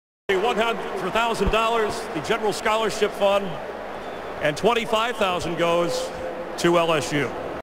Ohio State beats LSU at 2007 BCS Halftime Alumni Kickers
Alumni kicking field goals at halftime is the only thing Ohio State is going to beat LSU or any other SEC team at. Congratulations to Ohio State!  Alumni kicking is the only bright spot during the BCS Championship Game because you suck so bad!
LSU beat Ohio State worse than the score indicates.
1.  Power - LSU's offensive and defensive lines overpowered Ohio State.
2.  Team Speed - LSU played man defensively and blew by Ohio State's defensive man coverage.
3.  Outcoached - Most embarrasing for Ohio State.  LSU's Gary Crowder called plays in situations that totally confused OSU.  See other vids (when they are all up).